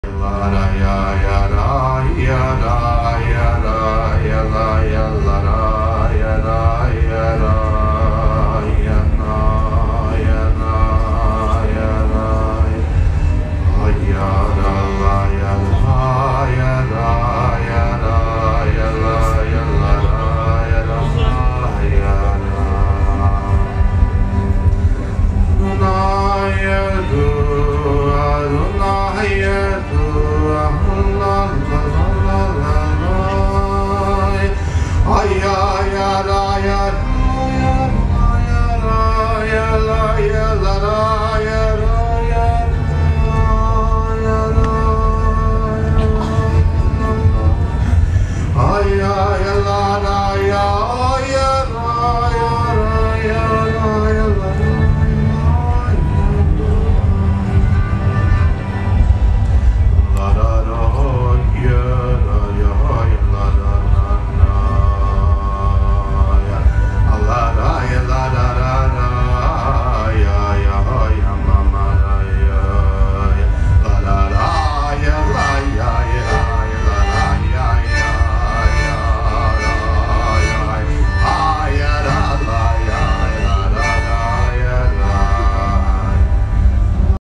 Pohoda 2010 Chasidske piesne - Dunaj
Chasidske piesne (Chasidi songs) perform at Pohoda festival 2010 in Trencin, Slovakia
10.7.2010